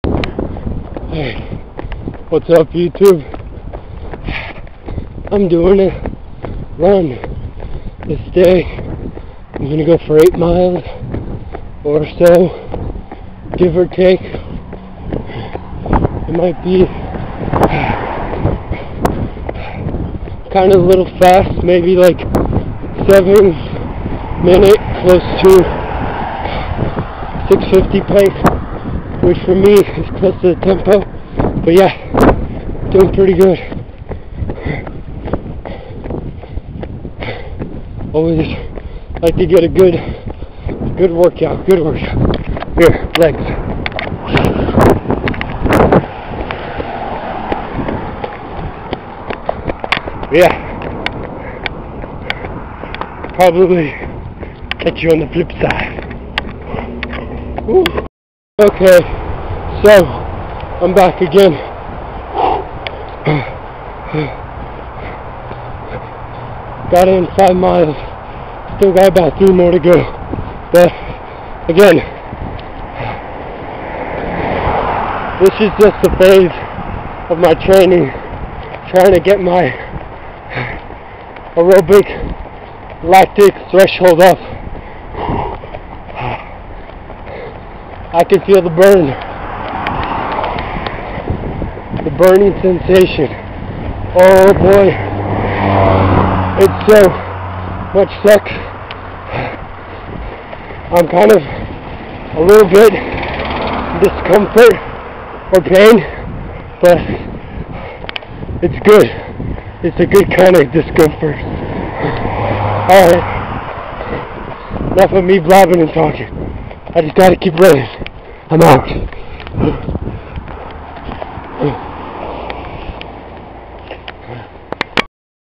8 Mile Tempo Run
Before the storm I got a run in